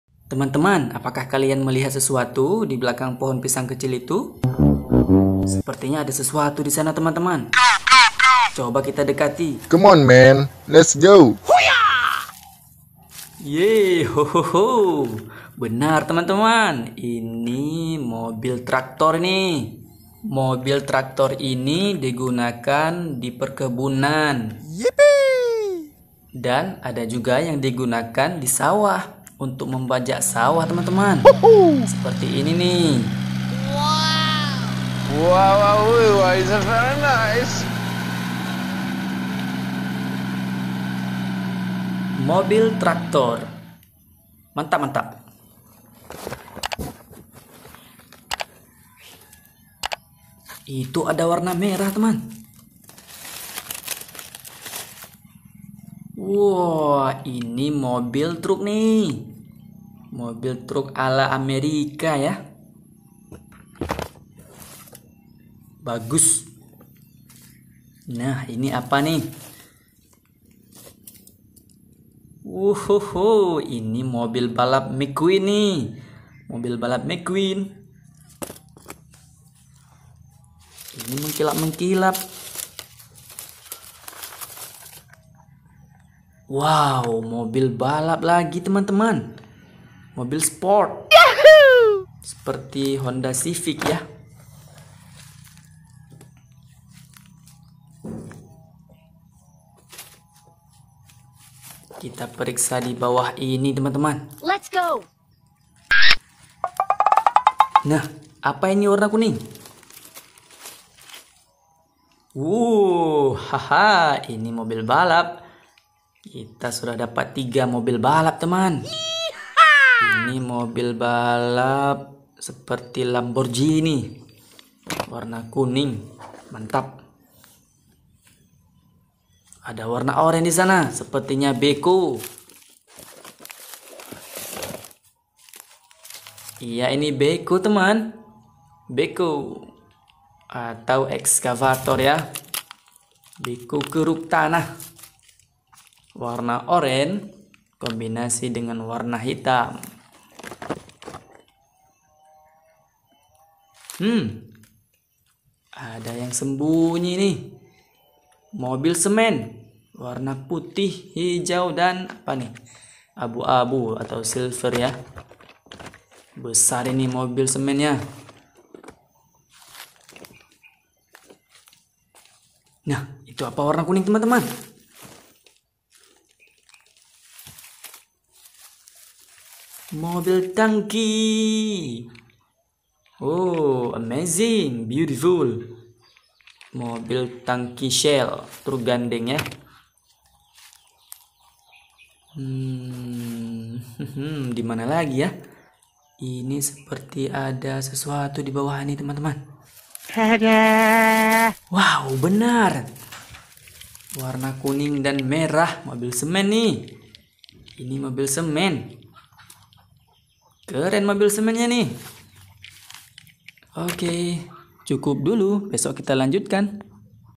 Mobil Mobilan - Traktor, Mobil Truk Amerika, McQueen, Mobil Balap, Beko, Truk Molen, Mobil Tangki
Melihat mobil - mobilan
Traktor, mobil truk amerika, McQueen, mobil balap, beko, truk molen atau truk semen, dan mobil tangki gandeng.